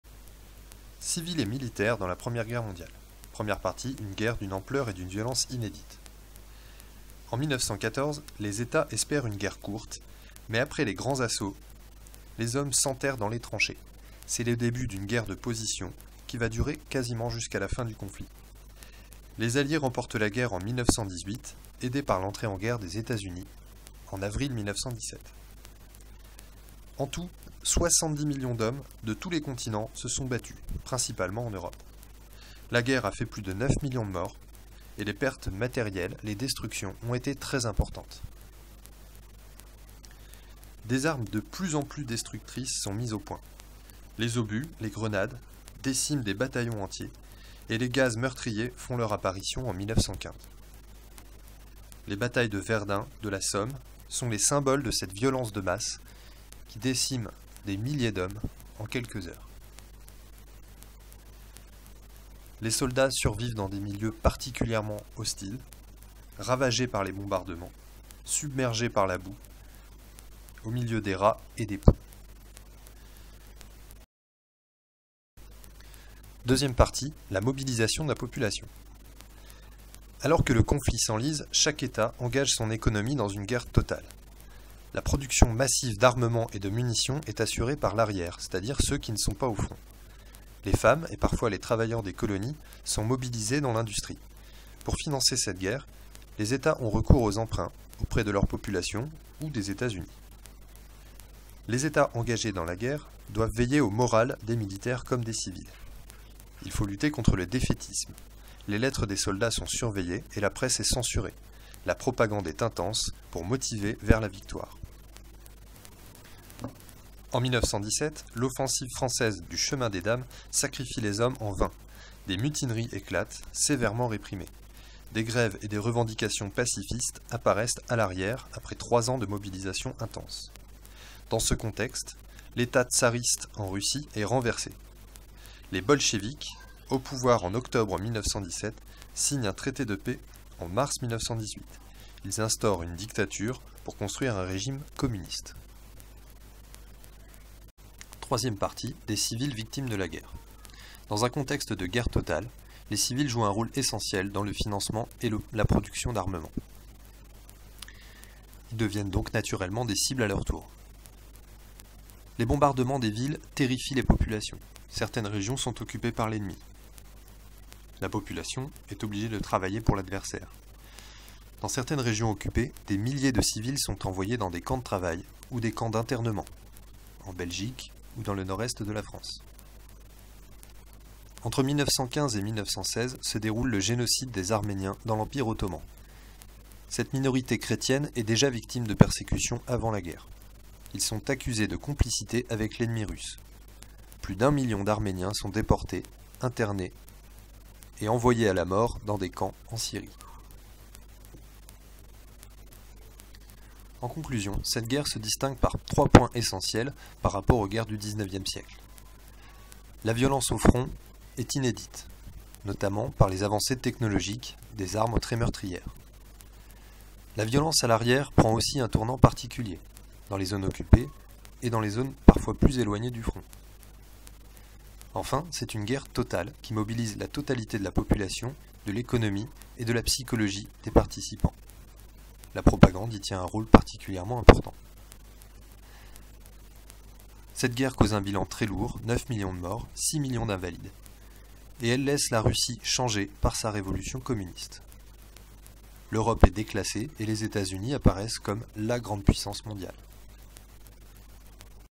Civils et militaires dans la première guerre mondiale
Comment la guerre de 14-18 a-t-elle profondémment touché les civils et les militaires ? Les Etats et les Sociétés ?